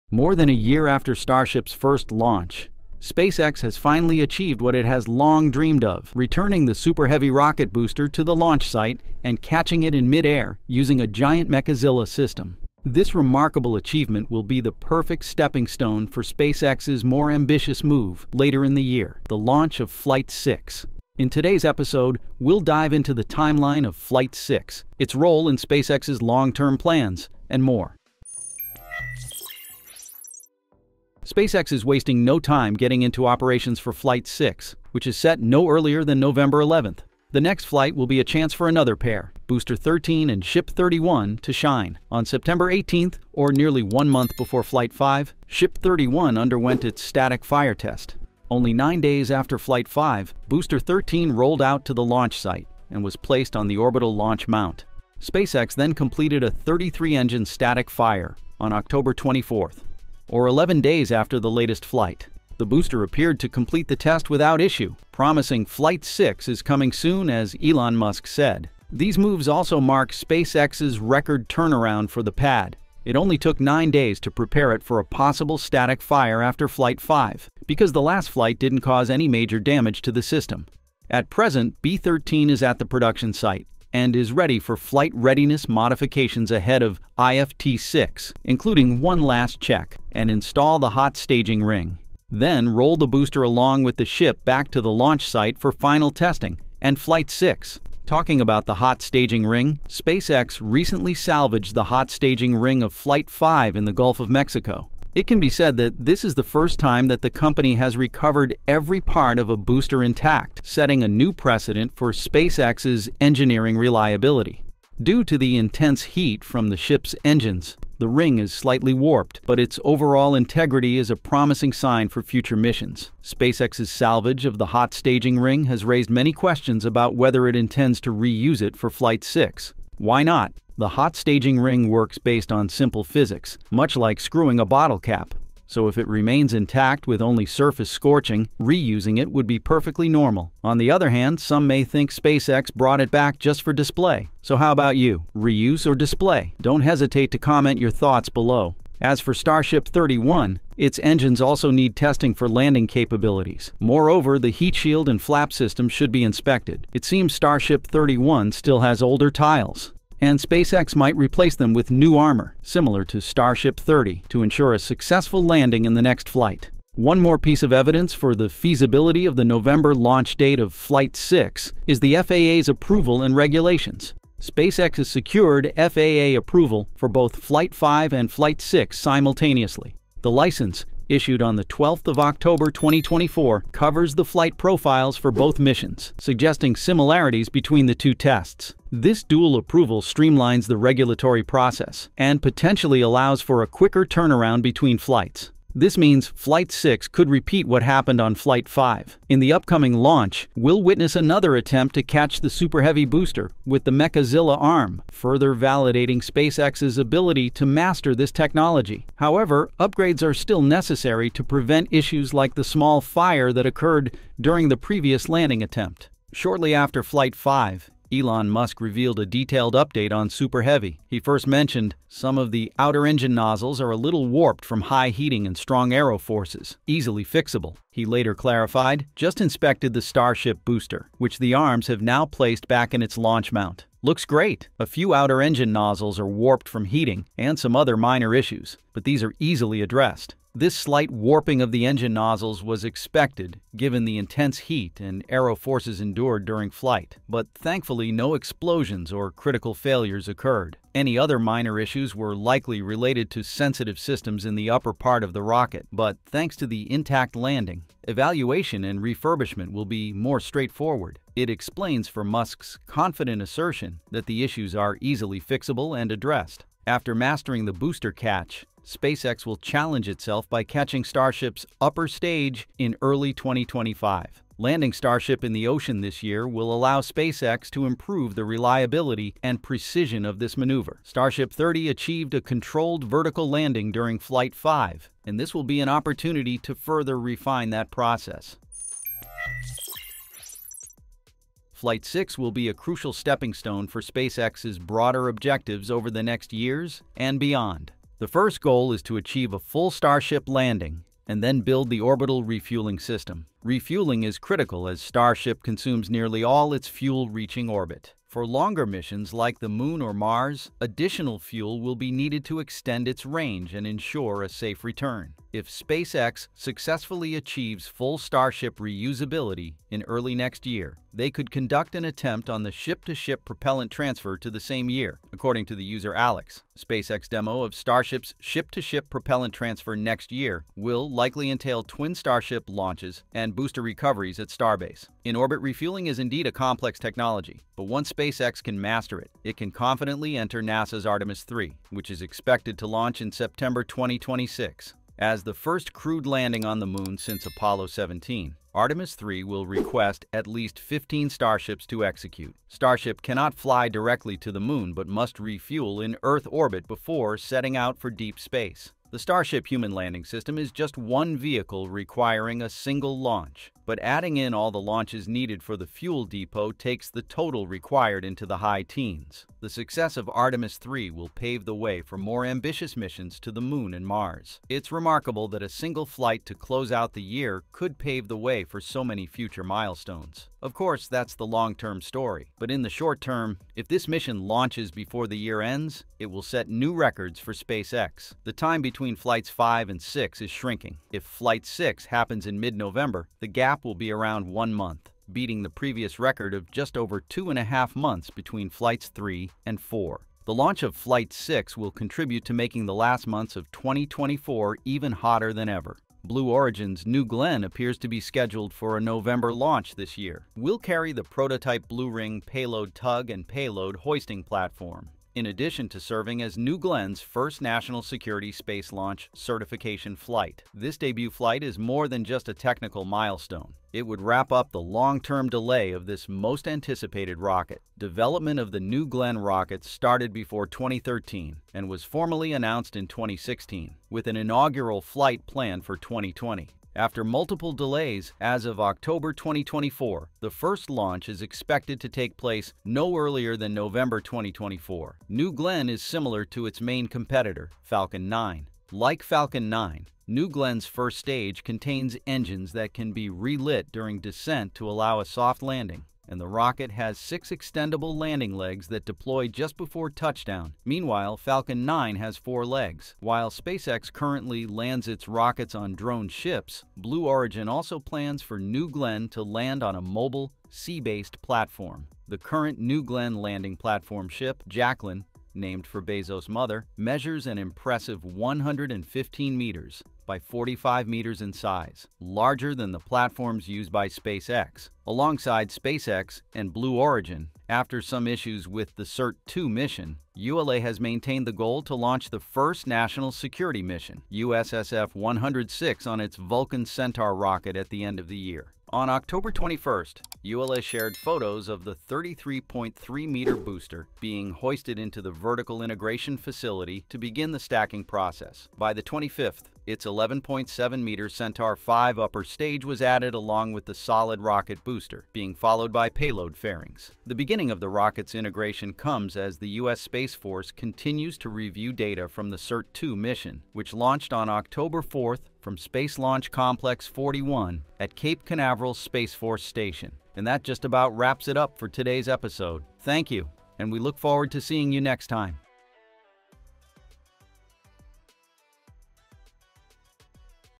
SpaceX's Wild Plan for Starship Launch Again in NOVEMBER Shocked Whole Industry...
intro 0:00
Quick turnaround 0:28
Crucial stepping stone 4:48
outro 8:50
1) SOURCES OF THUMBNAIL
2) SOURCES OF IMAGES AND VIDEOS
More than a year after Starship's first launch, SpaceX has finally achieved what it has long dreamed of:
Returning the Super Heavy rocket booster to the launch site and catching it in mid-air using a giant Mechazilla system.
This remarkable achievement will be the perfect stepping stone for SpaceX's more ambitious move later in the year: the launch of Flight 6.
In today’s episode, we’ll dive into the timeline of Flight 6, its role in SpaceX’s long-term plans, and more.
SpaceX is wasting no time getting into operations for Flight 6 which is set no earlier than November 11. The next flight will be a chance for another pair: Booster 13 and Ship 31, to shine.
On September 18 or nearly one month before Flight 5, Ship 31 underwent its static fire test.
Only nine days after Flight 5, Booster 13 rolled out to the Launch Site and was placed on the Orbital Launch Mount. SpaceX then completed a 33-engine static fire on October 24, or 11 days after the latest flight. The Booster appeared to complete the test without issue, promising Flight 6 is coming soon as Elon Musk said.
These moves also mark SpaceX's record turnaround for the pad. It only took 9 days to prepare it for a possible static fire after flight 5 because the last flight didn’t cause any major damage to the system.
At present, B13 is at the production site and is ready for flight-readiness modifications ahead of IFT-6 including one last check, and install the hot staging ring. Then roll the booster along with the ship back to the launch site for final testing and flight 6!
Talking about the hot-staging ring, SpaceX recently salvaged the hot-staging ring of Flight 5 in the Gulf of Mexico.
It can be said that this is the first time that the company has recovered every part of a booster intact, setting a new precedent for SpaceX’s engineering reliability. Due to the intense heat from the Ship’s engines, the ring is slightly warped but its overall integrity is a promising sign for future missions.
SpaceX’s salvage of the hot-staging ring has raised many questions about whether it intends to reuse it for Flight 6. Why not? The hot-staging ring works based on simple physics, much like screwing a bottle cap, so if it remains intact, with only surface scorching, reusing it would be perfectly normal. On the other hand, some may think SpaceX brought it back just for display. So, how about you? Reuse or Display?